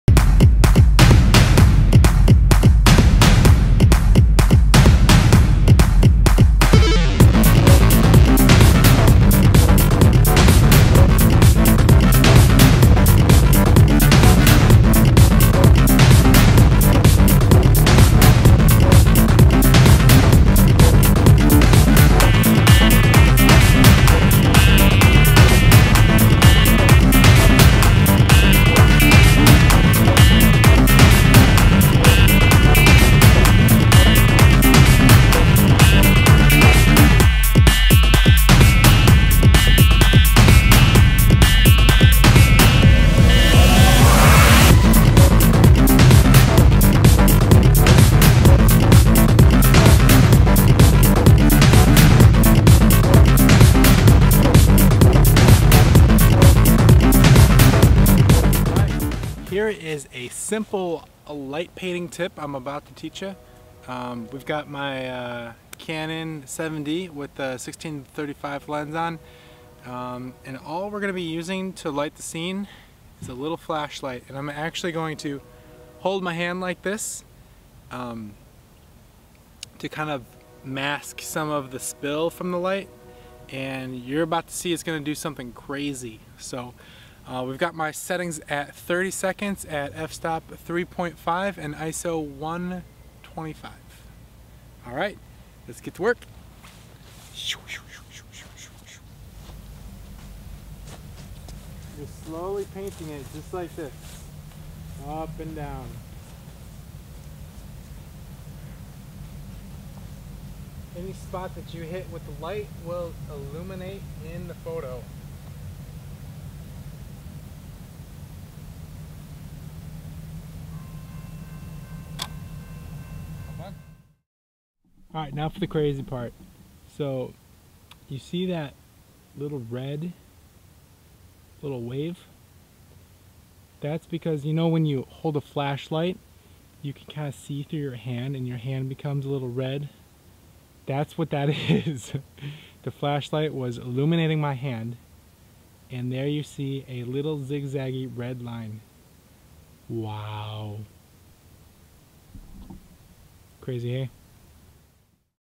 Light Painting with a Flashlight
If you're new to light painting check out how to paint with a simple flashlight!